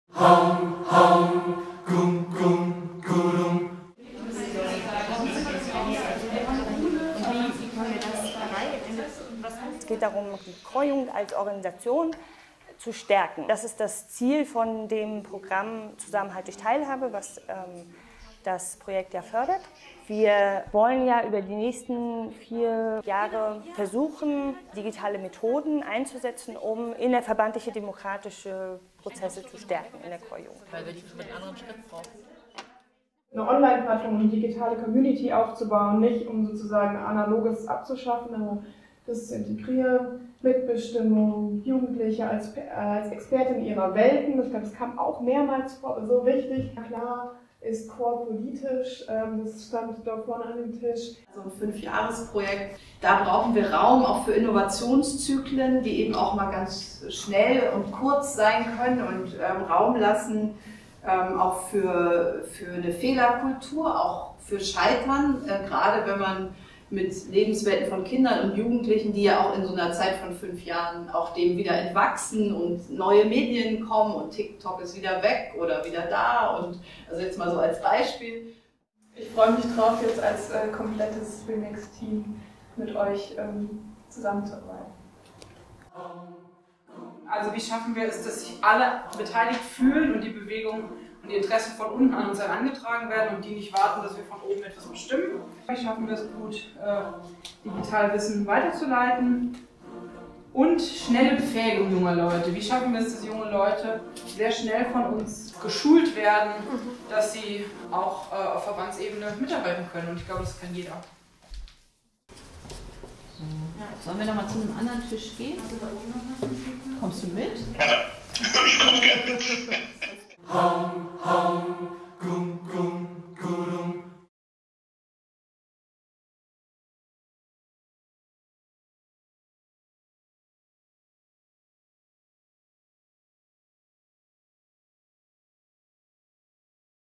re:mix - Worum es geht
Na klar ist Chor politisch! Es geht darum, die Chorjugend als Organisation zu stärken.

Das Kooperationsprojekt re:mix - Jugend singt und mischt sich ein, ist ein Modellprojekt der deutschen Chorjugend und mediale pfade im Rahmen des Bundesprogrammes Zusammenhalt durch Teilhabe der Bundeszentrale für politische Bildung. Es geht darum, gemeinsam innovative digitale Methoden zu entwickeln und zu nutzen, um Teilhabe und innerverbandliche Demokratie zu stärken.

Wir sind überzeugt, dass es eine emanzipatorische Praxis mit digitalen Medien und neuen Technologien braucht, um eine offene, gerechte und solidarische Gesellschaft zu gestalten. Mit zeitgemäßen Bildungsformaten befähigen wir Menschen, mit uns gemeinsam dieses Ziel zu erreichen.